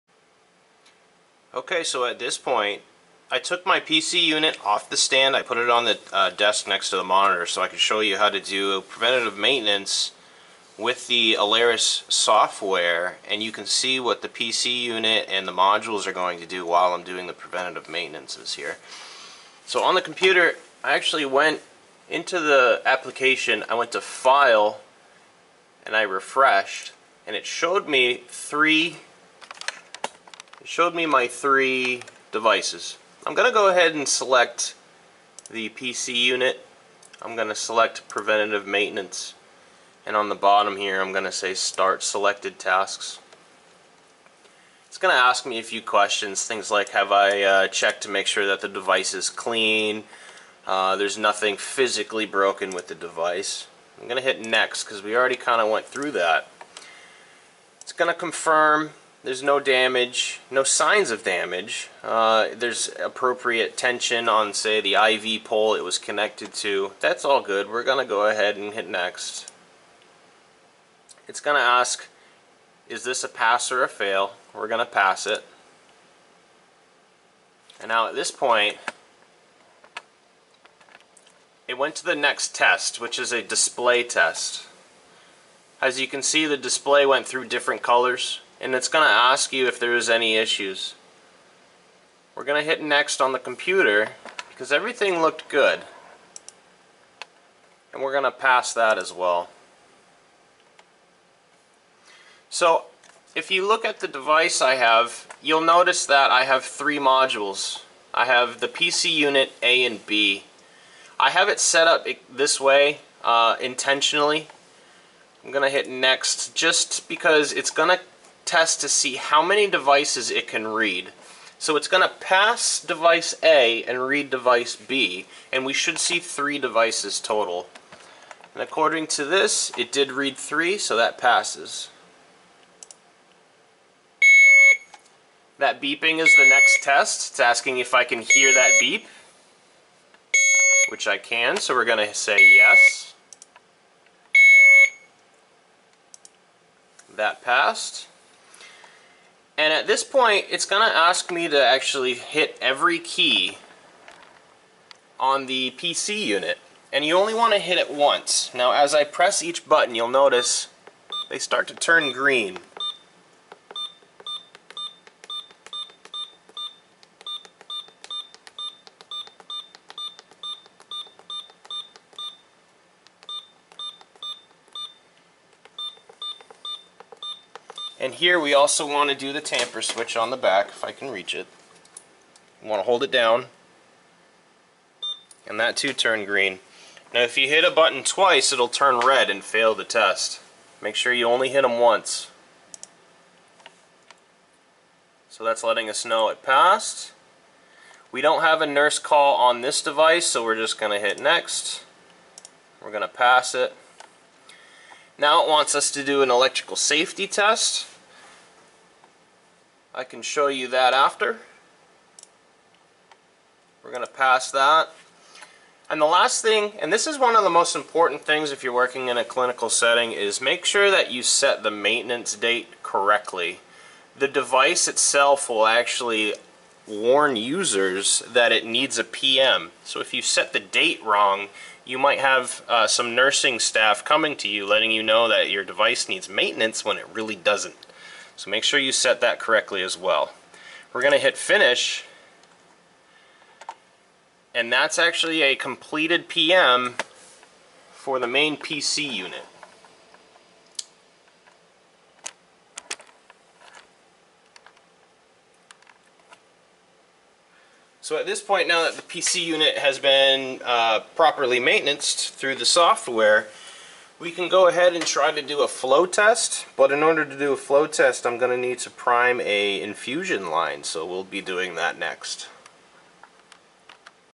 Infusion Pump: Hospira Alaris Preventative Maintenance Software
Video shows how to step through the Hospira Alaris Preventative Maintenance Software.